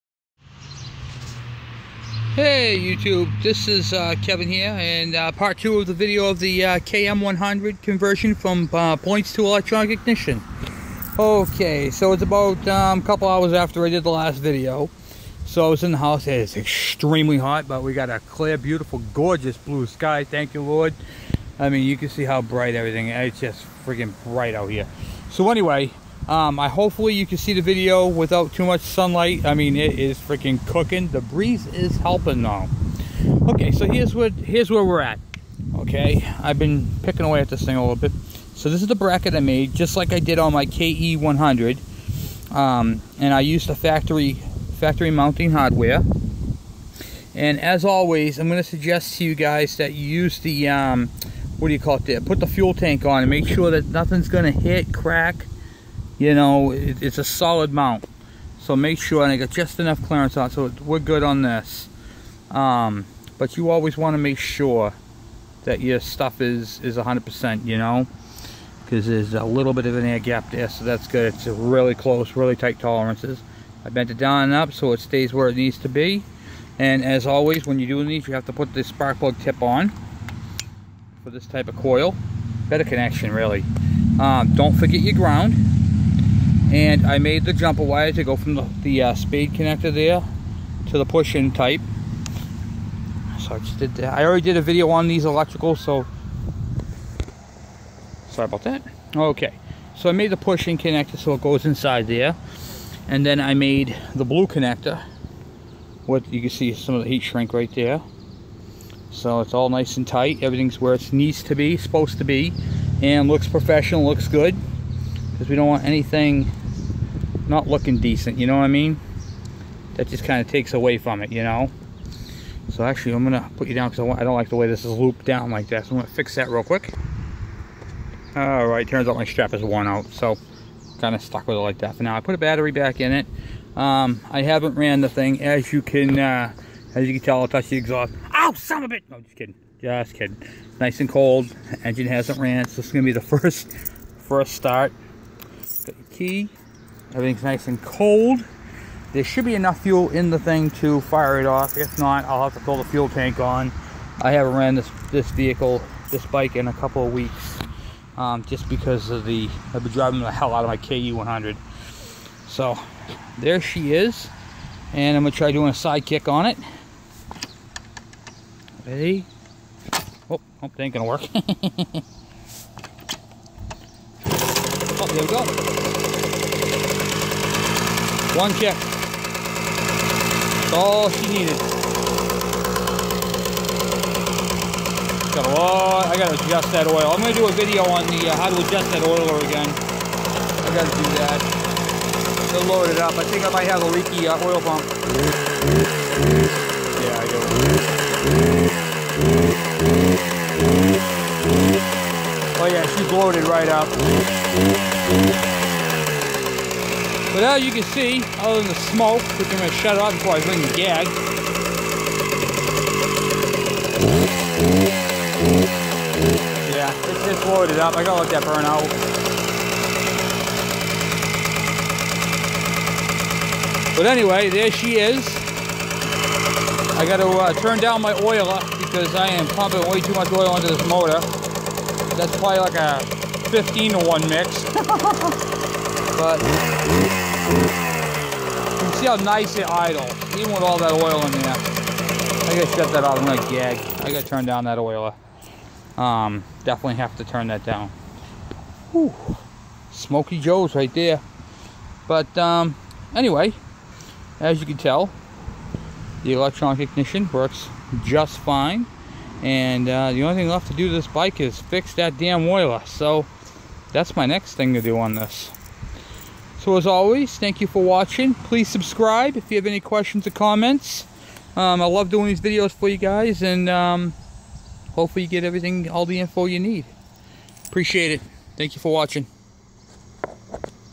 KM100 points to electronic ignition part 2
this video is part 2 of the 1979 KM100 points to electronic ignition using 2000 KE100 parts this conversion is the same for g3 g4 KE KD KM KC KH 80cc 90cc 100cc models all years shows tools needed and how to